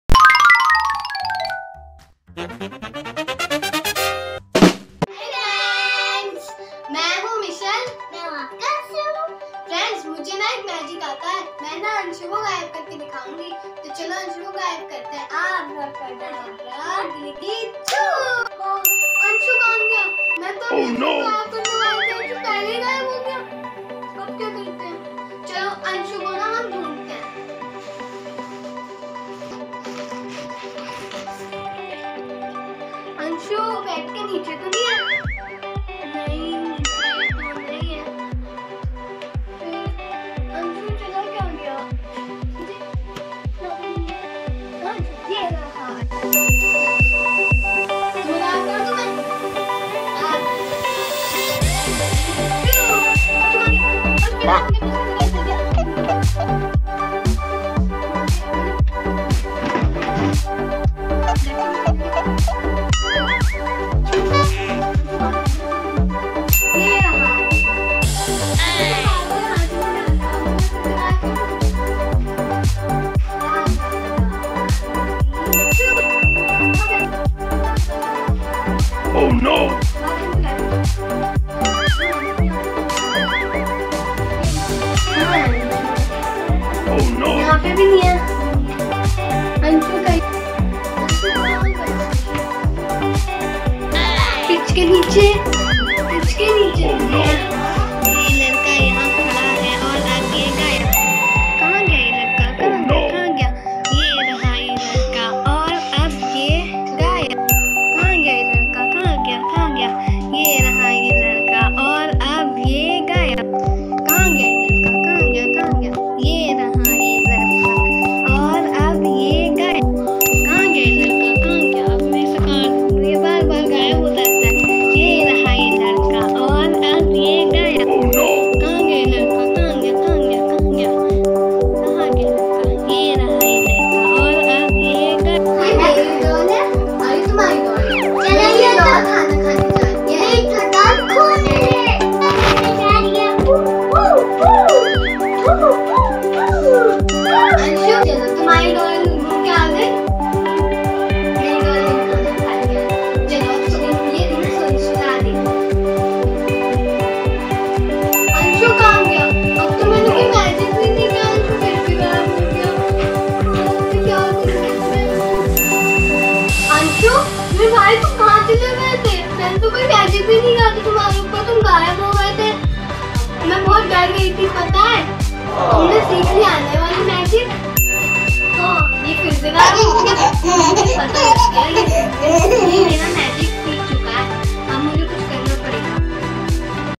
January 19, 2021
hi friends welcome to abi and anshu show pata friends aj is video me anshu banega chhota jadugar Vedio ko like share or subscribe zaroor karna to Vedio ka maza lete or haan friends comments me batana chhote jadugar ne apni sister ko kaise pareshan kiya apko kaisa laga bye bye